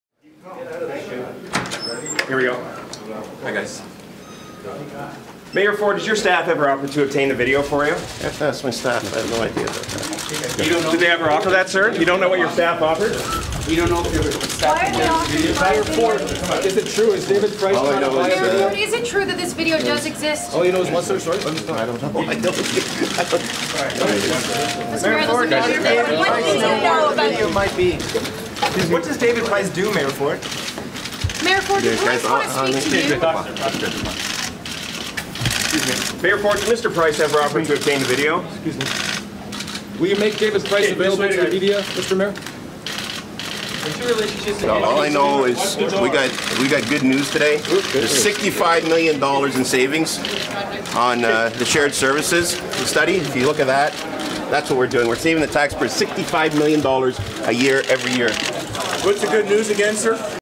Latest in Rob Ford saga
On Tuesday morning Toronto Mayor Rob Ford was questioned whether or not his staffers attempted to obtain a video that allegedly shows him smoking crack.